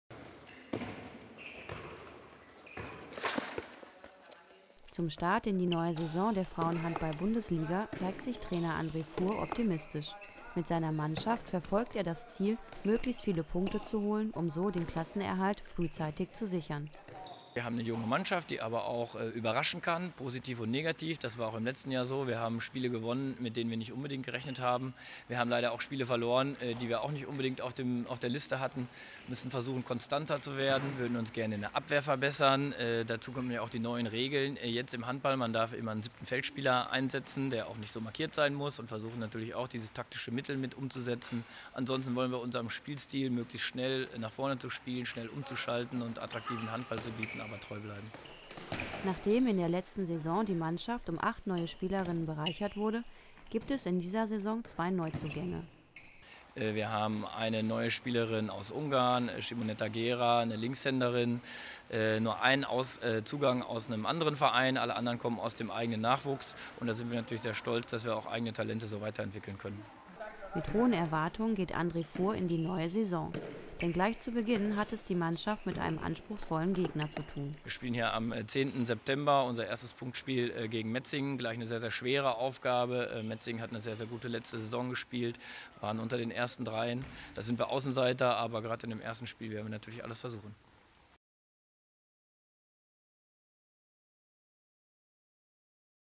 Saisonauftakt HSG Blomberg-Lippe
Beschreibung: In knapp einem Monat beginnt die neue Saison der Frauen Handball Bundesliga. Trainer André Fuhr blickt zuversichtlich in die neue Saison.